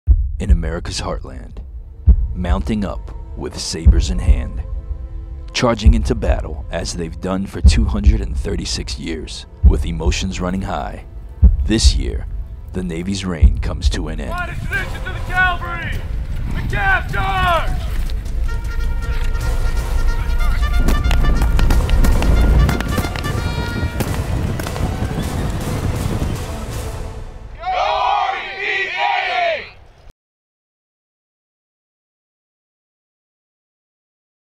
2011 Army/Navy Spirit Video-1st Infantry Division
This year the Navy's reign comes to an end. This Army-Navy spirit video was submitted by the 1st Infantry Division.